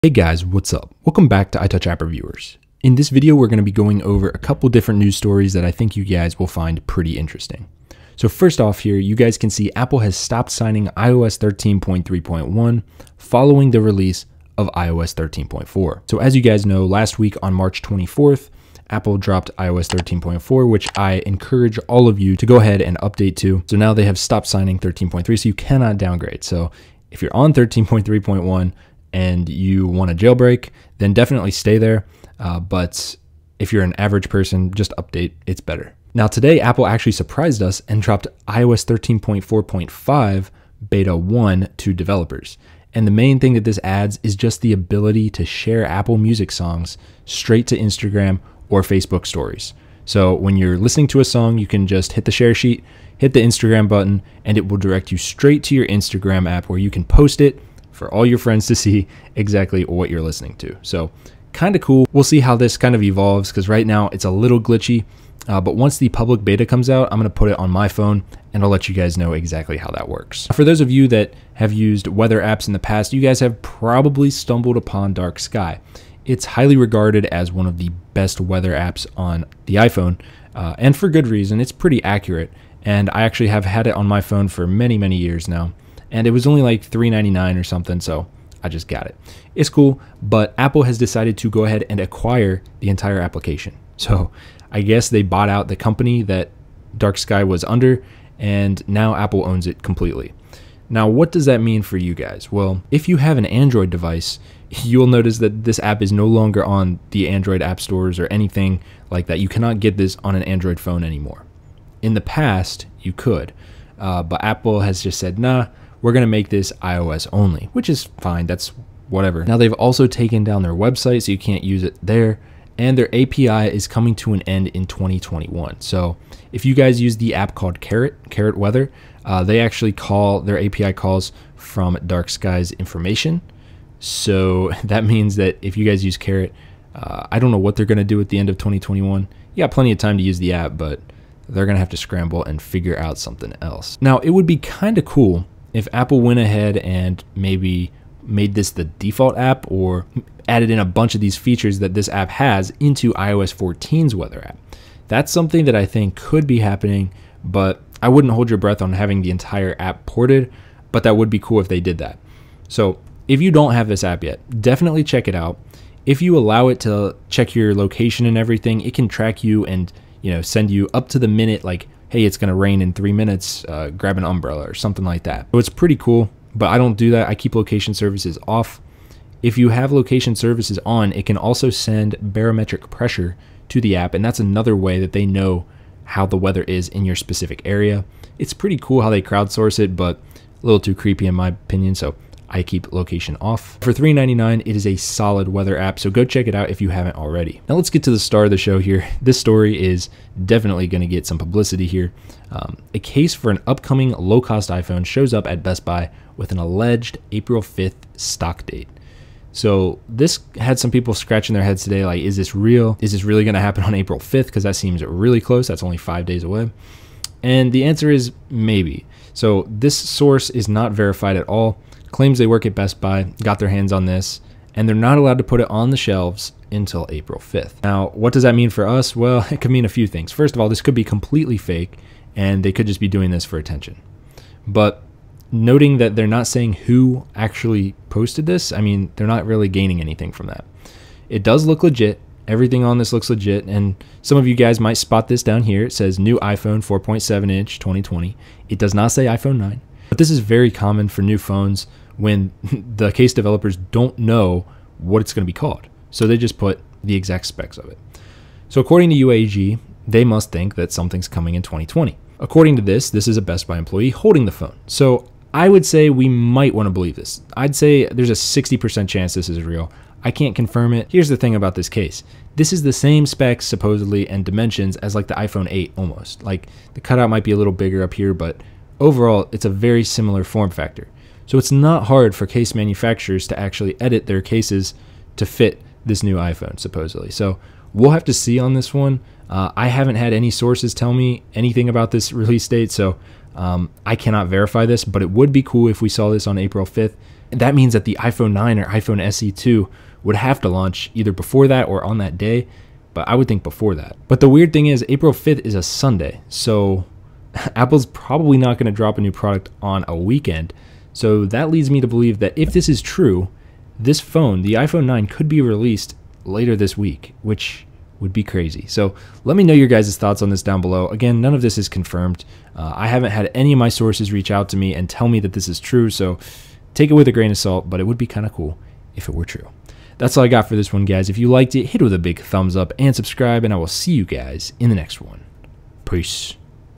iPhone 9 RELEASE THIS WEEK? // Leaked iPhone 9 Case + Dark Sky Acquired by Apple
A Best Buy employee has leaked a brand new iPhone 9 case to the public. Does this mean the iPhone 9 / iPhone SE 2 will release this week?
Apple also acquired Dark Sky today, which is well regarded as  one of the best weather apps on the iOS App Store. iOS 13.4.5 was also released today to developers via Developer Beta 1.